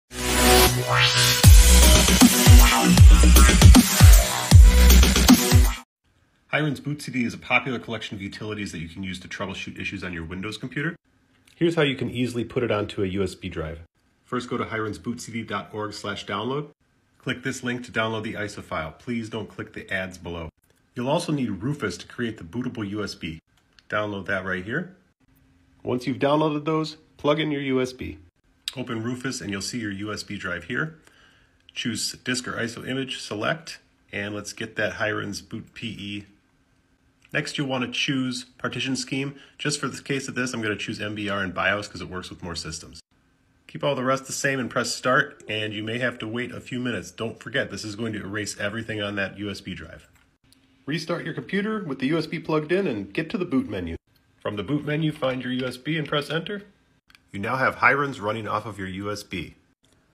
Hiren's BootCD PE for repairing your Windows 10
Hiren's Boot CD is an all-in-one bootable rescue disc solution that contains a load of useful Windows repair tools you'll want to have when you can no longer boot into your PC. Imagine issues like hard drive failure, virus infections, partitioning, password recovery, and data recovery